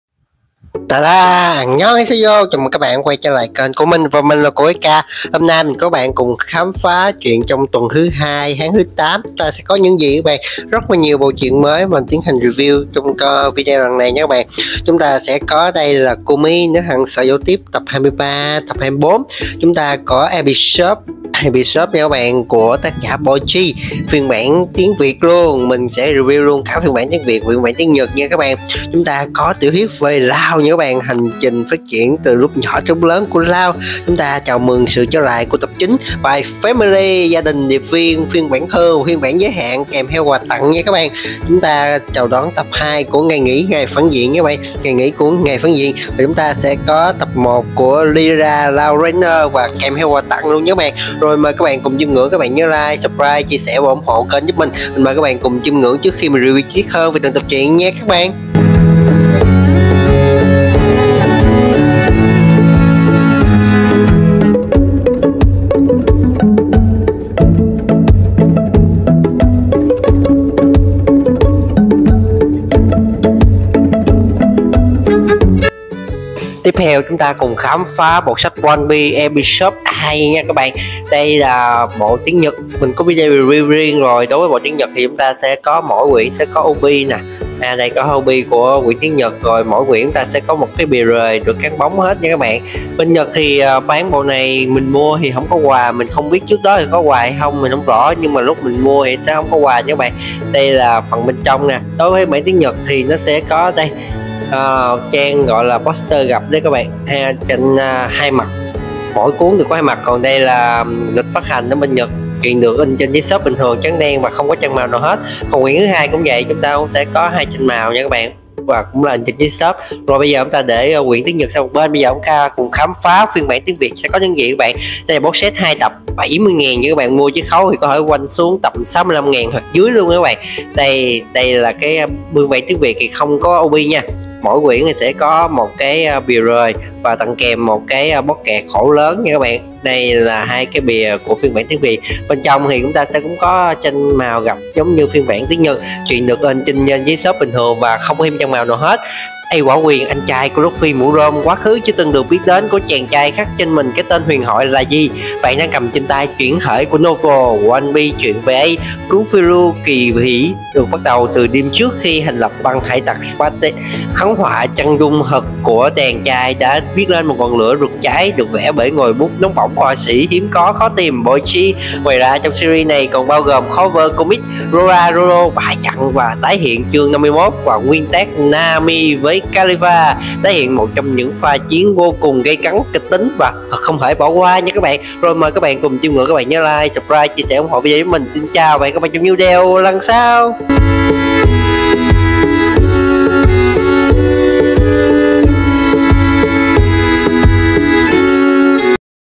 [Review #826] OnePiece Episode A-Tập 1+2 Kèm PVC Card
Bộ Sách One Piece - Episode A - Tập 1 + Tập 2 (Bộ 2 Cuốn)

Ace Hỏa Quyền, anh trai của Luffy Mũ Rơm. Quá khứ chưa từng được biết đến của chàng trai khắc trên mình cái tên huyền thoại là…? Bạn đang cầm trên tay bản chuyển thể của Novel "ONE PIECE chuyện về Ace" - cuốn phiêu lưu kí vĩ đại bắt đầu từ đêm trước khi thành lập băng Hải tặc Spade, khắc họa chân thực chàng trai đã sống như một ngọn lửa rực cháy, được vẽ bởi ngòi bút nóng bỏng của họa sĩ hiếm có khó tìm Boichi!!

Ngoài ra series truyện này còn bao gồm Cover Comic: “Roronoa Zoro bại trận”, tái hiện chương 51 của nguyên tác và “Nami VS. Kalifa”, tái hiện một trong những pha chiến đấu gây vô cùng kịch tính, thực không thể bỏ lỡ!!